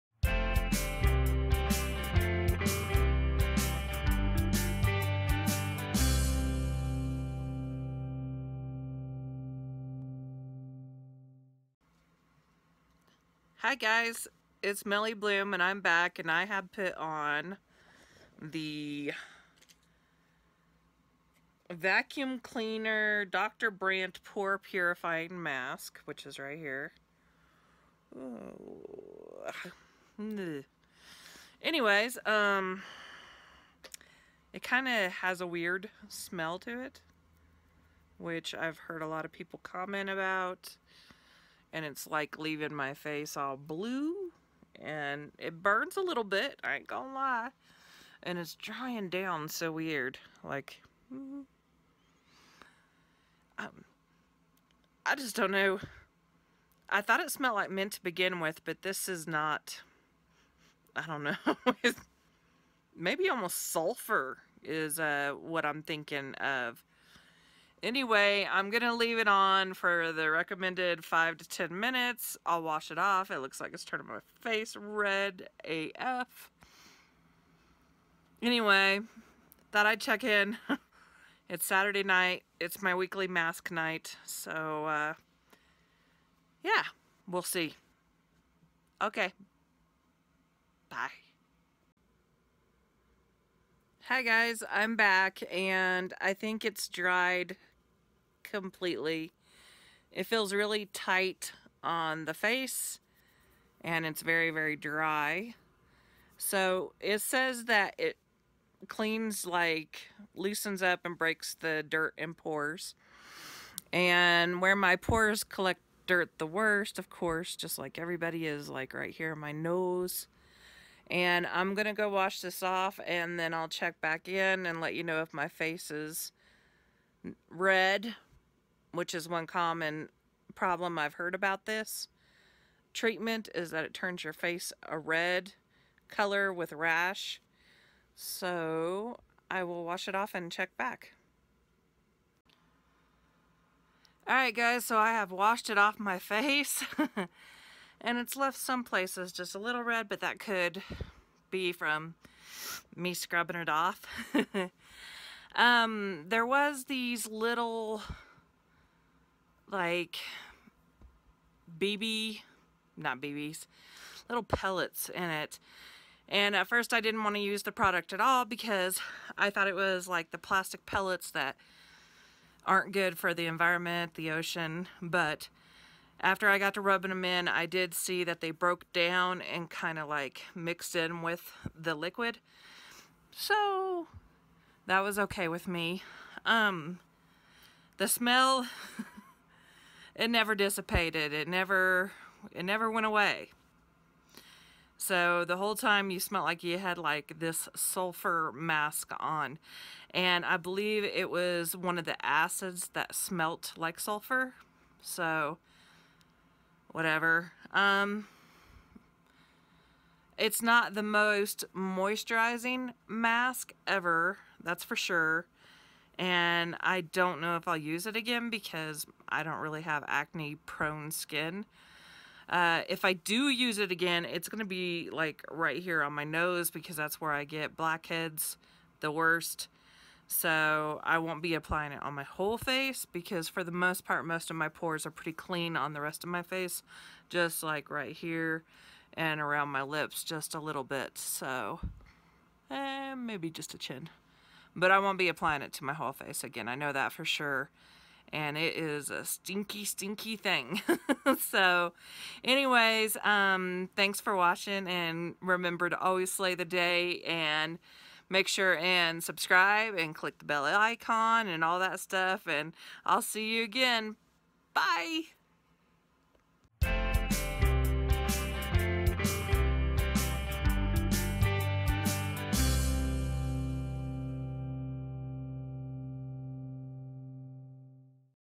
Smelly Mask Dr. Brandt vacuum cleaner
So like most I got this wonderful mask in Boxy Charm. Here is what I think about it .